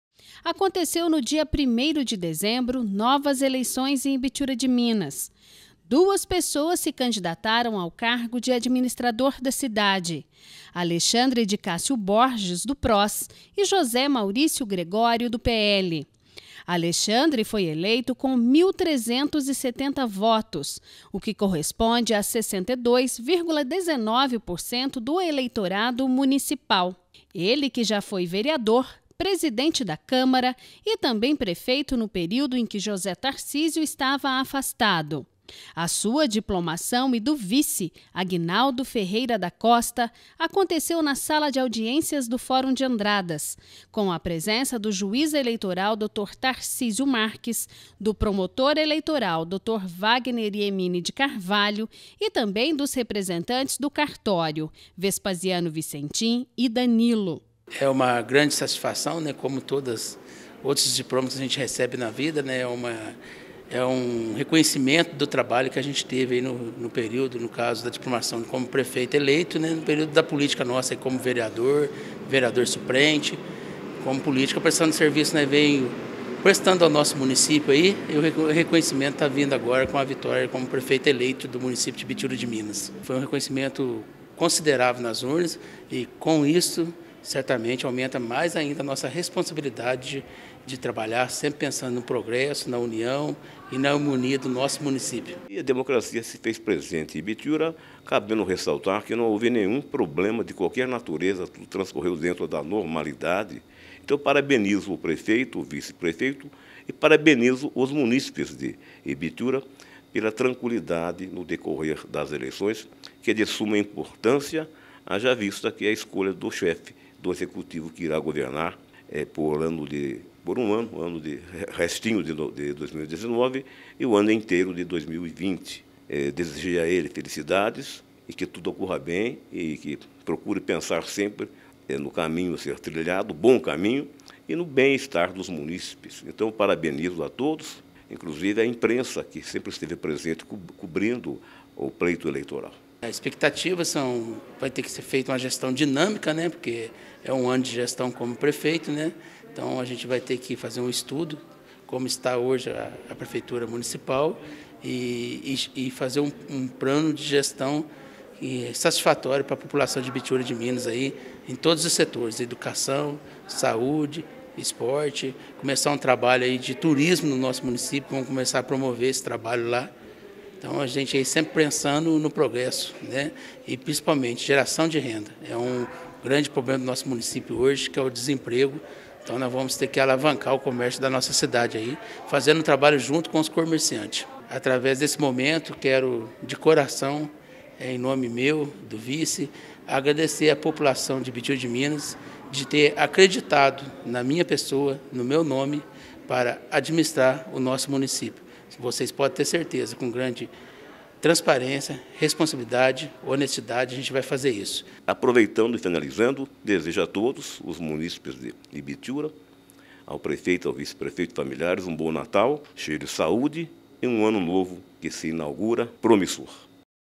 IBITIURA DE MINAS VAI TER DURANTE O ANO DE 2020 NOVOS GOVERNANTES
Ibitiura de Minas vai ter durante o ano de 2020 novos governantes. Alexandre de Cássio Borges e Aguinaldo Ferreira da Costa foram diplomados na tarde desta quinta-feira pelo juiz eleitoral da comarca de Andradas Dr. Tarcísio Marques. A solenidade de posse do prefeito e vice eleitos em Ibitiura de Minas aconteceu às 17h00 desta sexta-feira na câmara municipal. Boa sorte aos novos representantes do poder executivo.